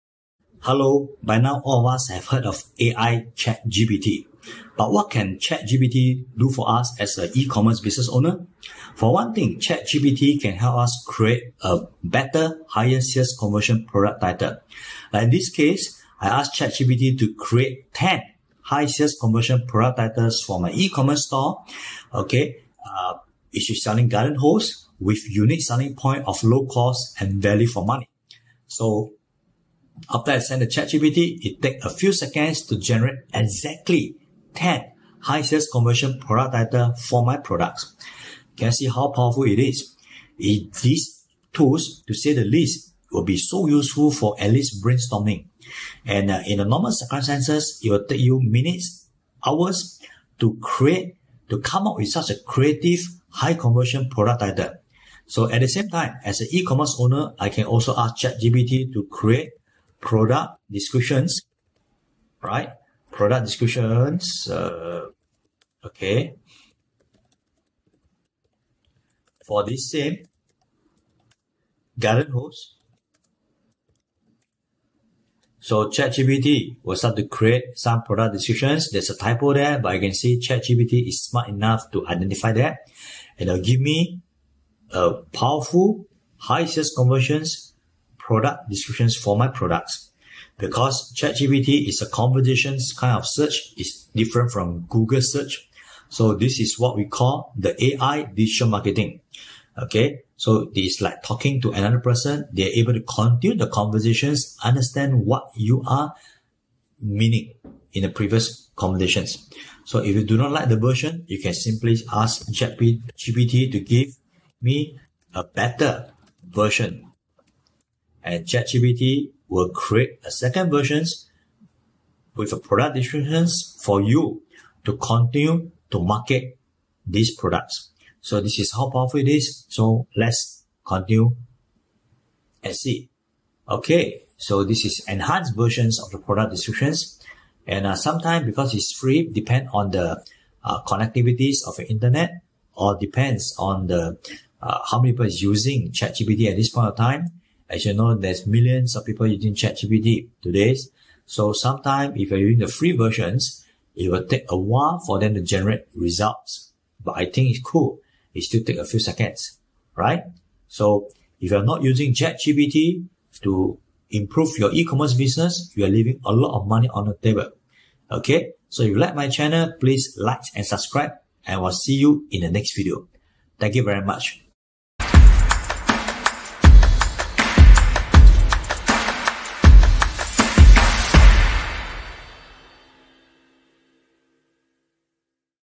How to Use ChatGPT to Scale Up Your Ecommerce Business in 2023
for more Free AI Digital Marketing insights and tips.